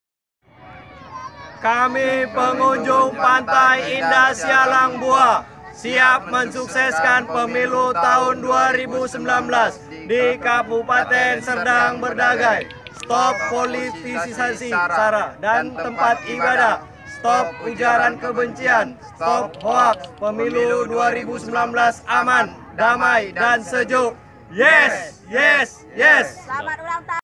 Liburan tanpa Hoax
Video deklarasi anti Hoax pengunjung pantai Indah Sialang Buah Kec. Teluk Mengkudu Kab. Serdang Bedagai dalam rangka mensukseskan Pemilu Damai 2019.